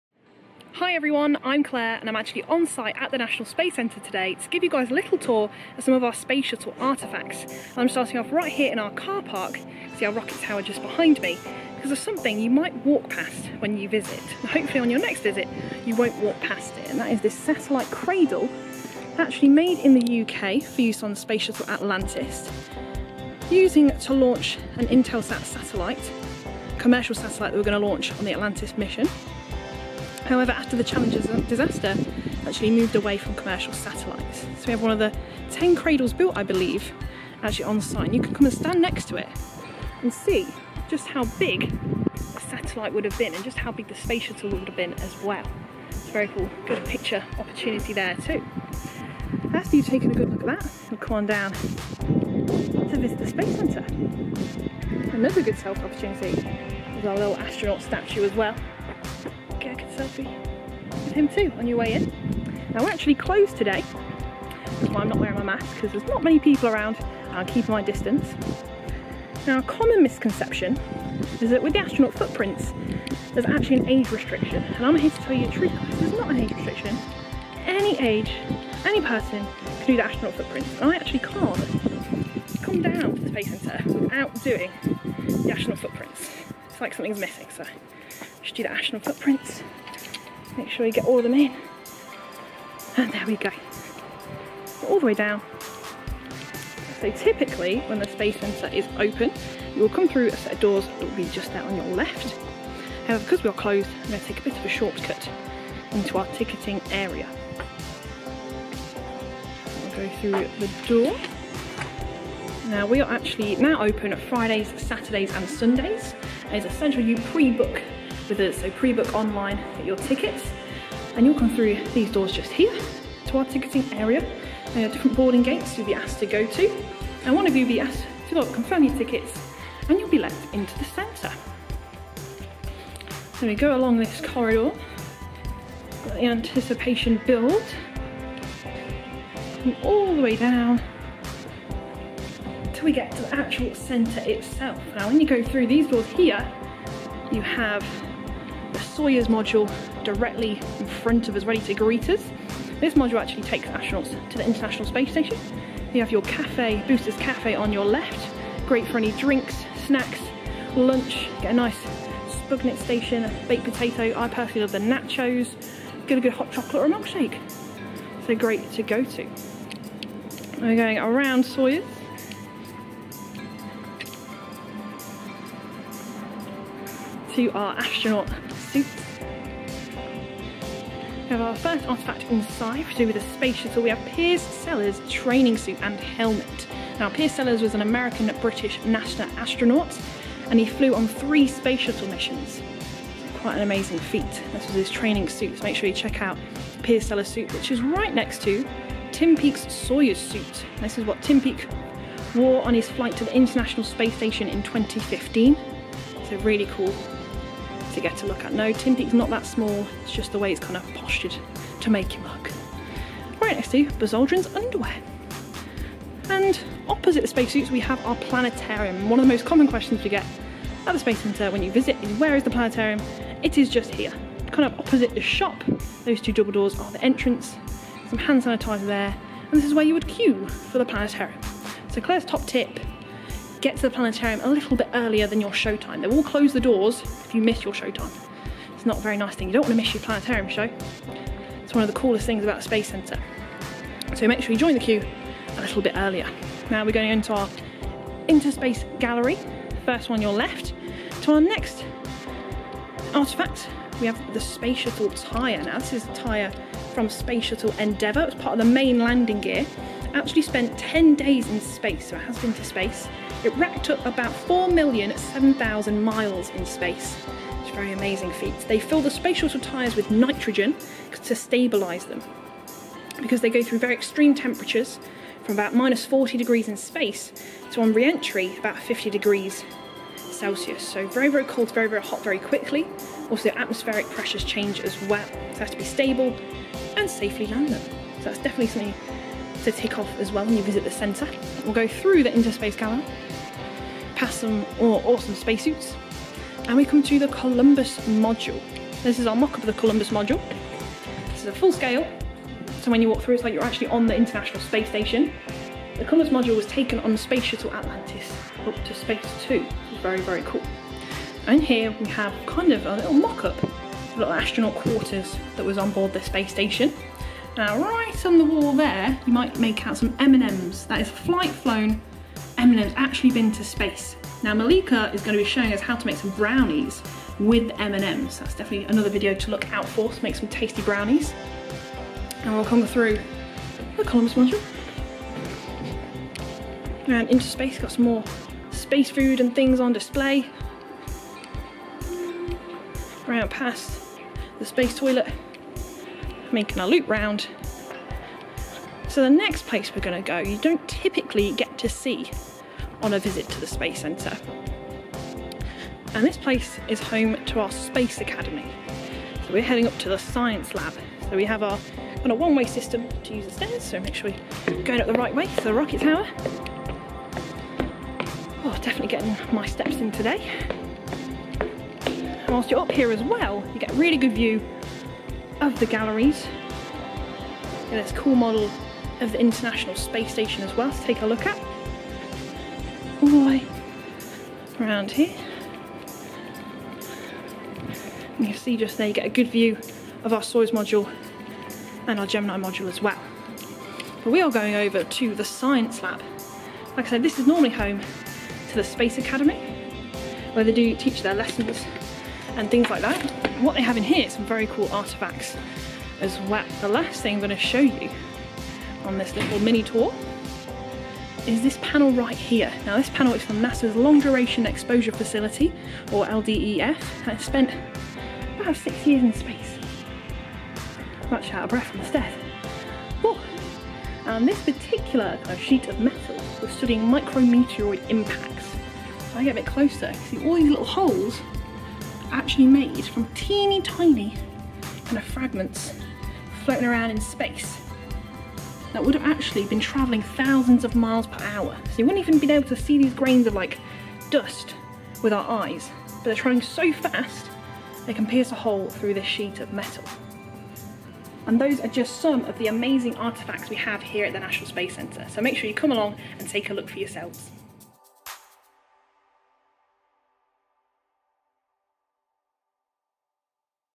Space Shuttle Artefacts Tour | National Space Centre
What NASA Space Shuttle artefacts are there at the National Space Centre? 🚀

Claire takes you on a tour to discover some amazing items in the collection that you might not have noticed before including spacesuits, space food and more.